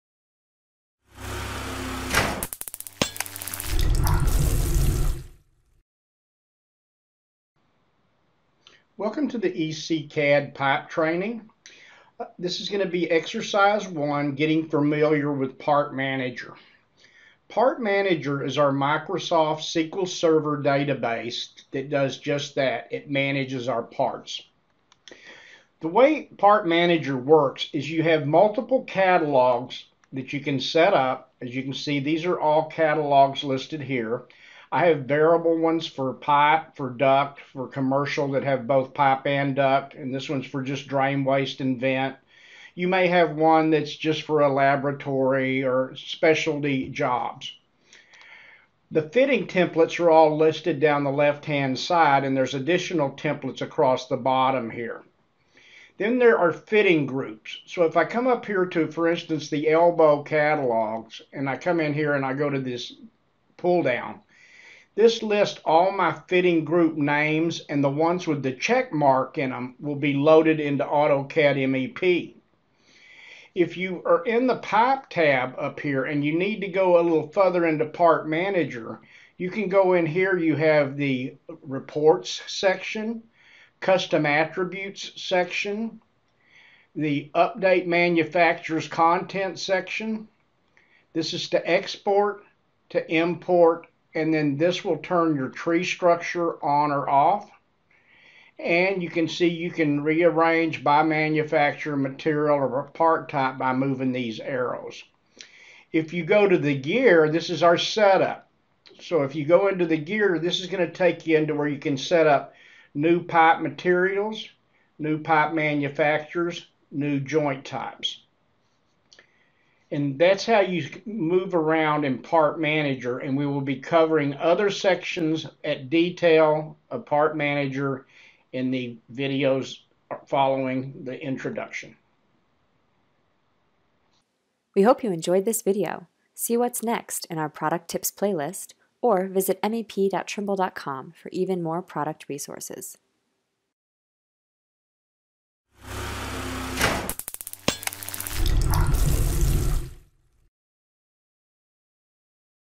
EC-CAD Pipe Training Exercise Part 1: Getting Familiar with Part Manager
This pipe training exercise video series will take you, step by step, through all the features and functions to model/draw more effectively with Trimble EC-CAD. In part 1 of this series, we introduce you to Part Manager.